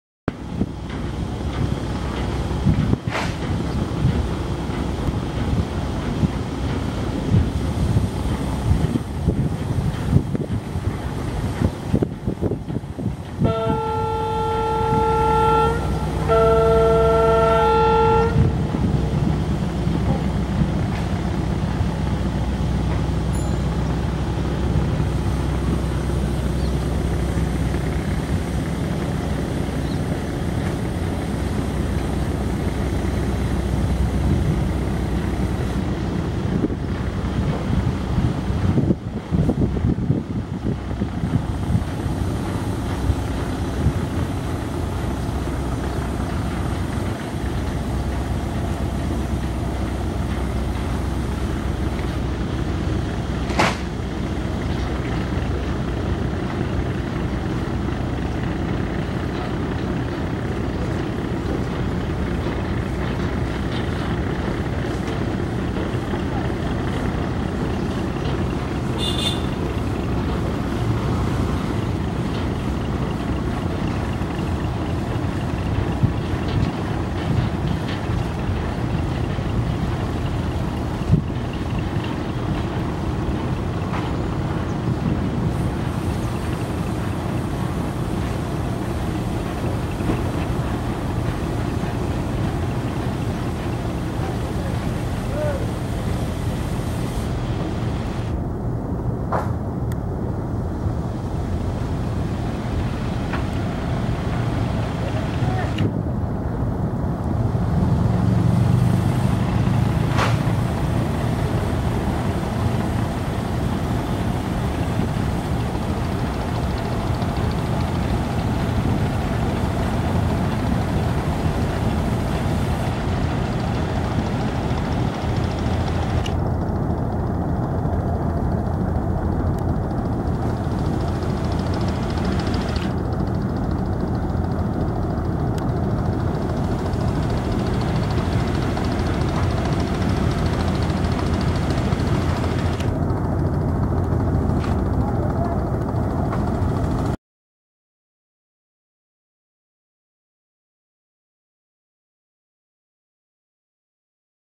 M.V. PRINCE AWLAD 2 Launch | Dhaka To Mridharhat Launch (Via Chandpur & Shoula) | M8893 Fastest Ship
M.V. PRINCE AWLAD 2 Launch | Dhaka To Mridharhat Launch (Via Chandpur & Shoula) | M-8893 Fastest Ship Of Juboraj River Service